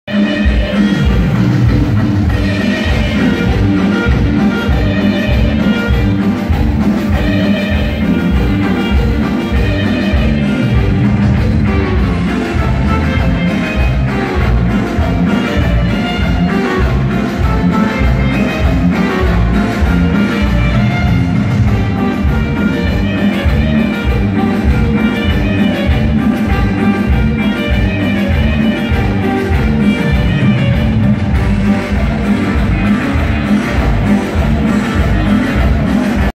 En el Escapade 2001 28 de Abril
Aquí con las amigas celebrando mi cumpleaños😊